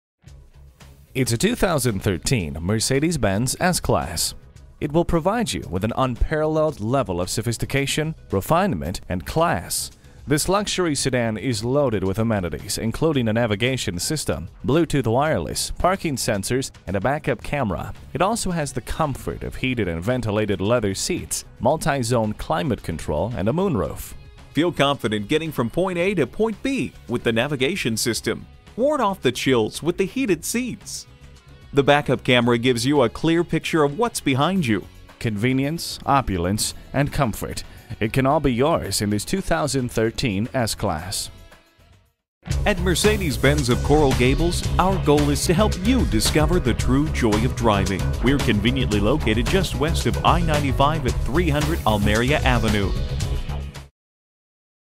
Certified 2013 Mercedes-Benz S-Class Miami FL Coral Gables, FL R2146 - SOLD
Year: 2013
Make: Mercedes-Benz
Model: S-Class
Trim: S550
Engine: 8 Cyl. 4.6L
Transmission: Automatic
Color: Diamond White Metallic
Interior: Beige
Mileage: 19446
Stock #: R2146
VIN: WDDNG7DBXDA530228

Address:
272 Valencia Ave